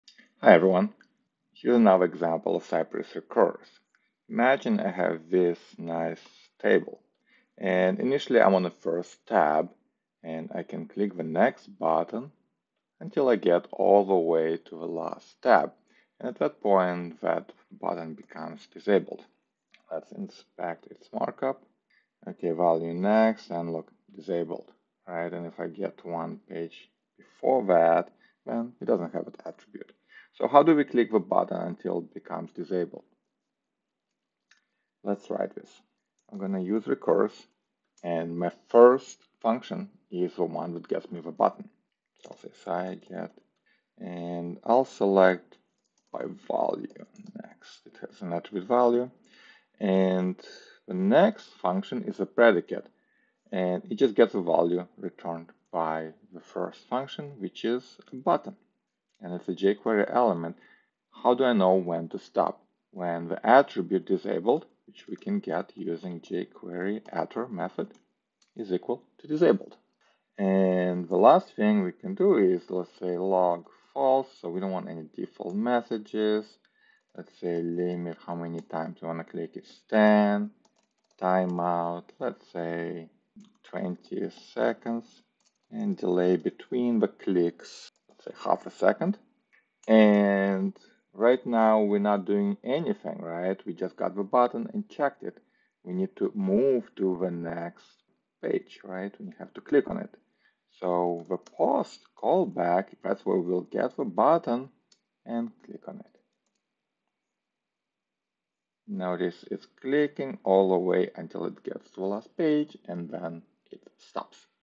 cypress-recurse: Click On The Button Until It Becomes Disabled
Another example: in a paginated table we need to go to the last page. We know we reached the last page when the "Next" button gets the attribute "disabled" We can check if an element has this attribute by using the jQuery attr method call.

recurse(
  function () {
    return cy.get('[value=next]')
  function ($button) {
    return $button.attr('disabled') === 'disabled'
    log: false,
    limit: 10,
    timeout: 20_000,
    delay: 500,
    post() {
      cy.get('[value=next]').click()

See the pagination-spec.js